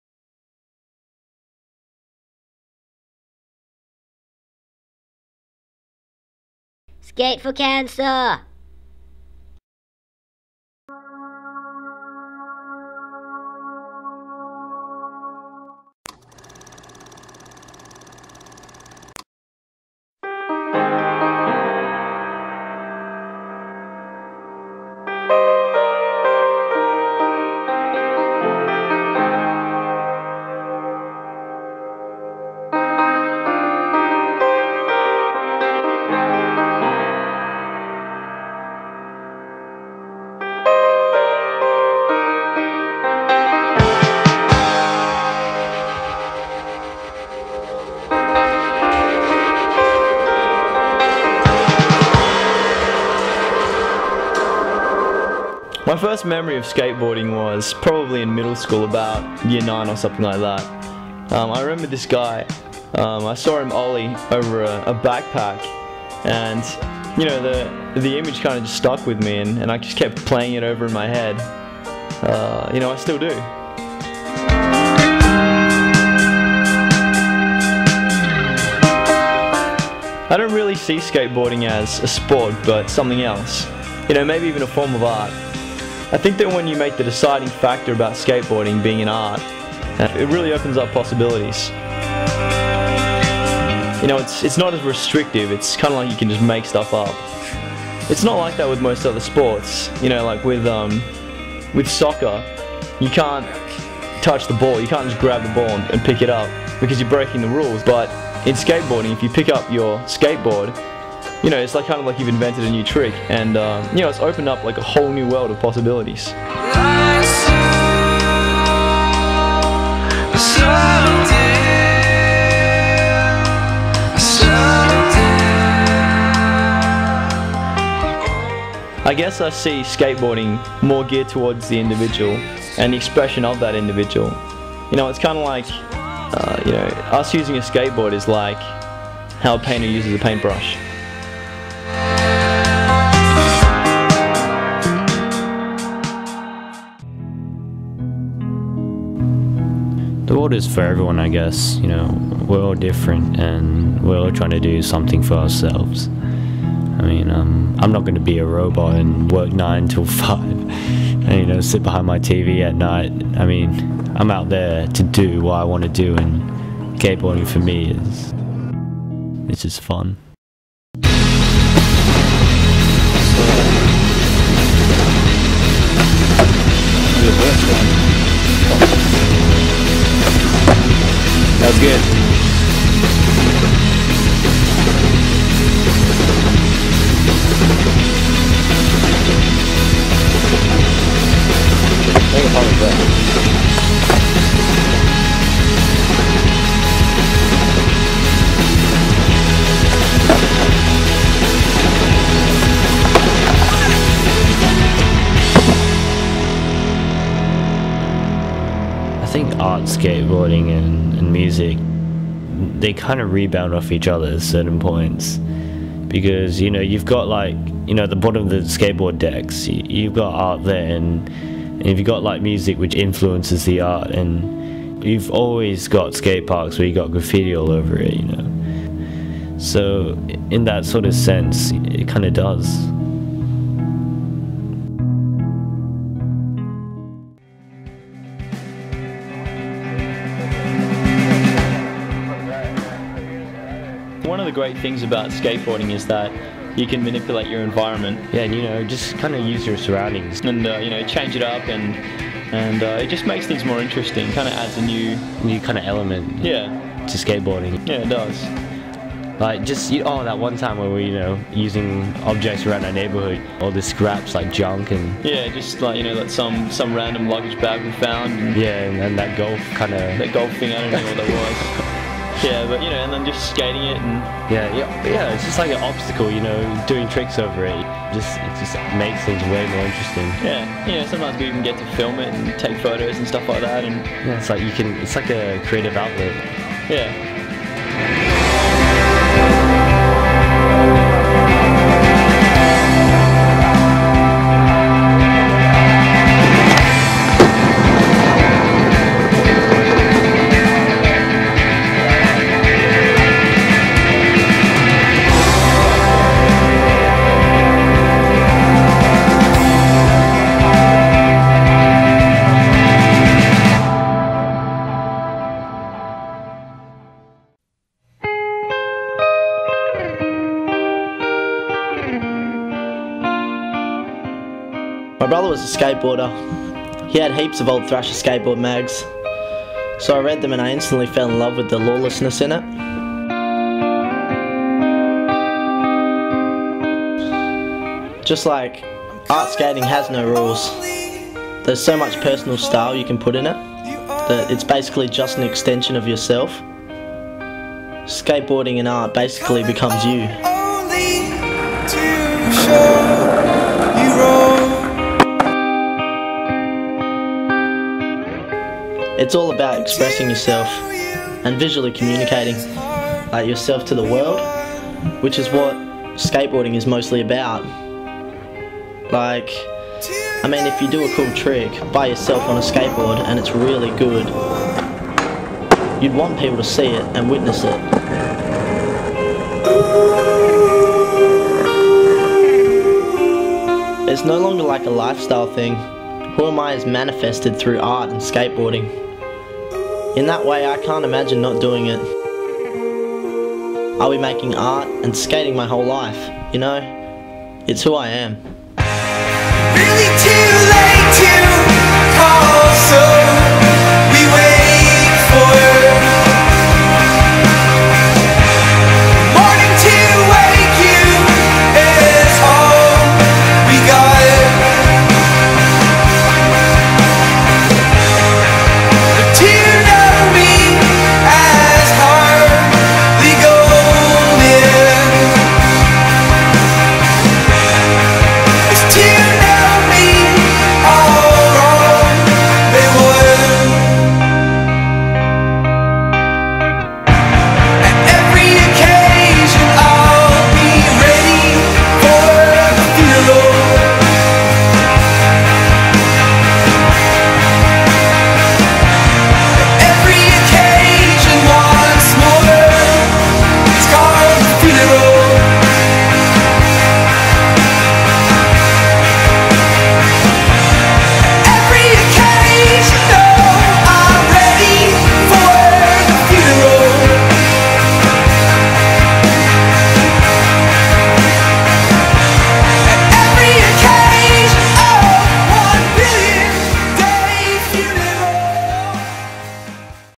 This is Who We Are - A Skateboard Documentary HD
''Skateboarding has always been much more than the progression of stunts performed on a skateboard. There is a whole creative mentality behind it all that has influenced out culture. Art being one of them.''  Lance Mountain

i did this for my media studies project. let me know what you think :)